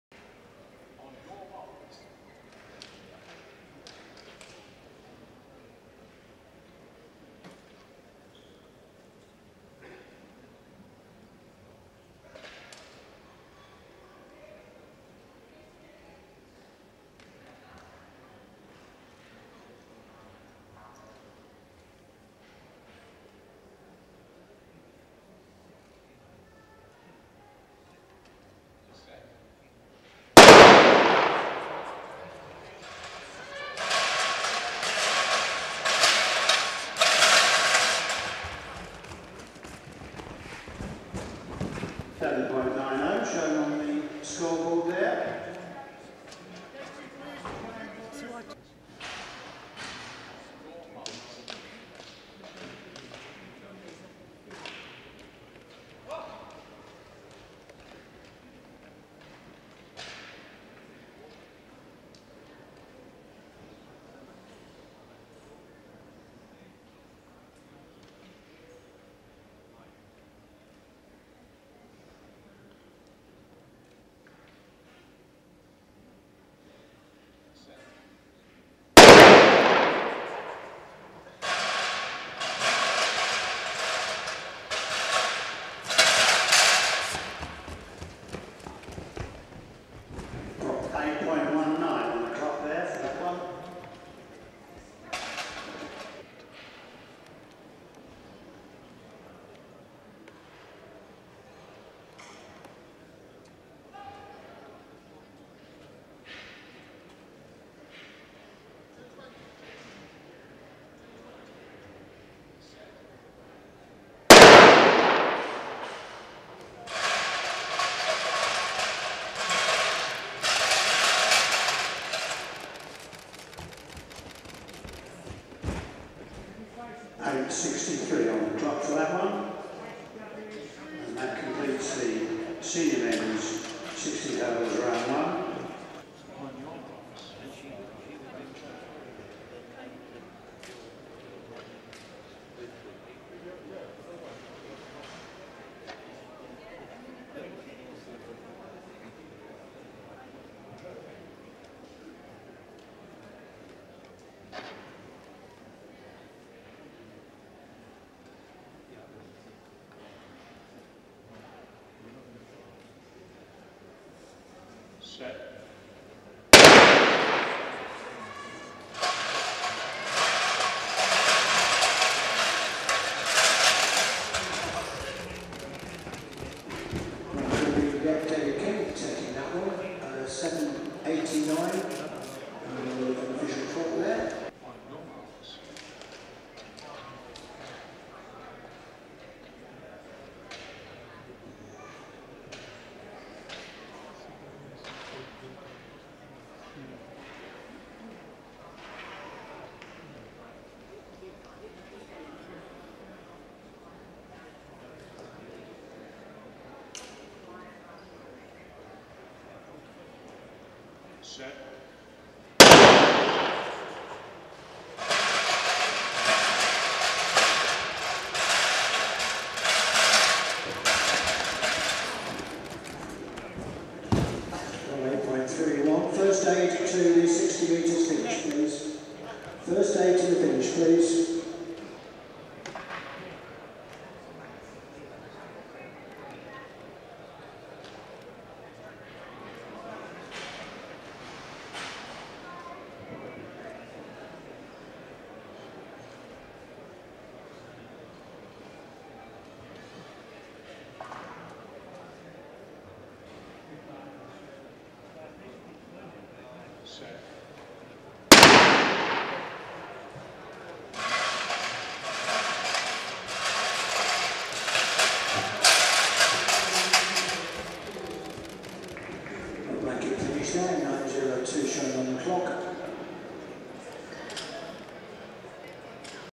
London Indoor Games 2015 - Senior Men 60m Hurdles
London Indoor Games
Senior Men 60m Hurdles (Race 1 - 3 Heats) & (Race 2 - 3 Heats)
Saturday 31st January 2015
Lee Valley Athletics Centre